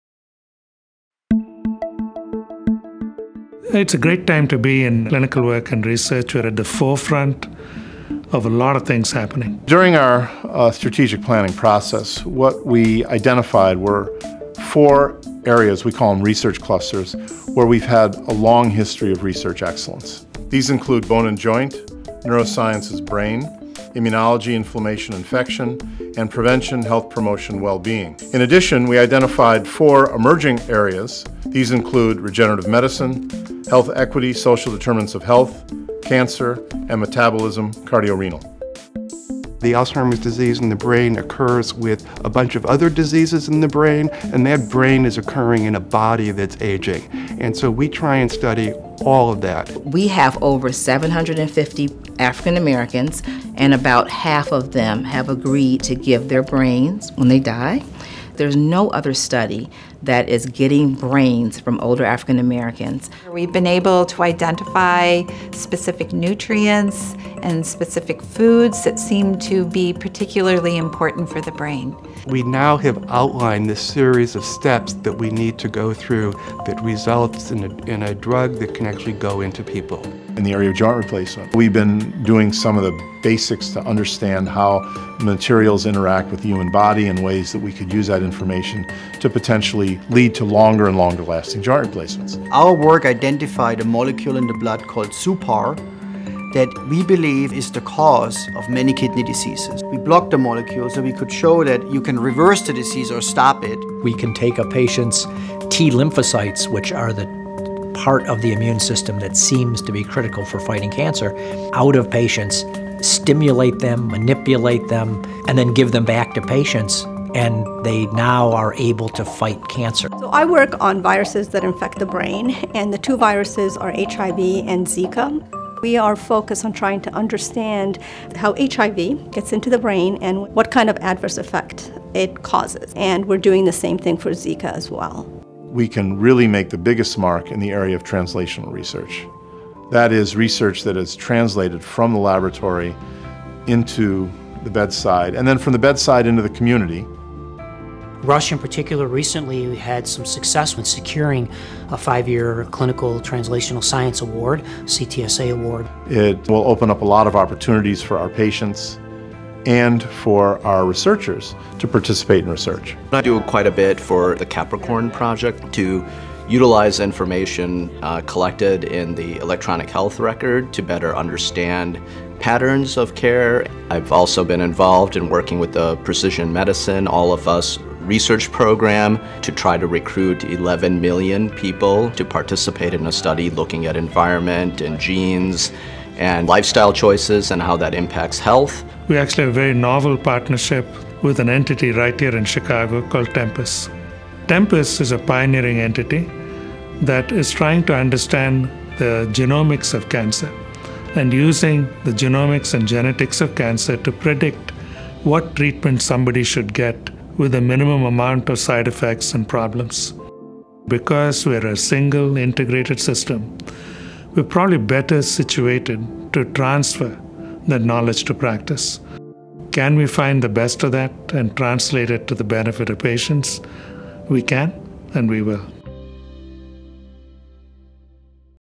Research at Rush
Learn how innovative research at Rush is helping to better understand and treat diseases and advance patient care.